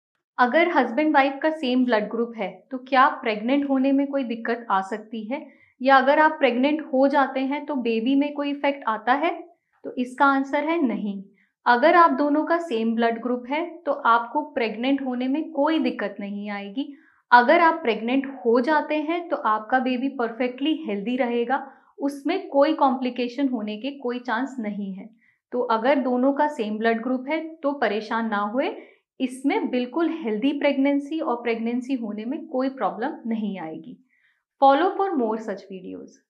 Does same blood group affect pregnancy?
We often come across concerns about the impact of same blood groups of partners on pregnancy and the unborn child. 🤔

But let me assure you, having the same blood group as your partner does NOT affect the pregnancy or the health of your baby. 🙅‍♀️🙅‍♂️

Blood groups are determined by the presence of certain proteins on the surface of red blood cells. If both partners have the same blood group, it means they share the same type of proteins on their red blood cells. This is not a problem, as the mother's immune system does not see the baby's blood cells as foreign and does noattack them. 🤝💕

In fact, it is only when the mother's blood group is Rh-negative and the baby's blood group is Rh-positive that complications can arise. In such cases, the mother's immune system may produce antibodies that attack the baby's red blood cells, leading to a condition called hemolytic disease of the newborn. This can be prevented with proper medical care and treatment. 💉💊

So, if you and your partner have the same blood group, there is absolutely nothing to worry about. You can rest assured that your baby will be completely safe and healthy. 🤗💙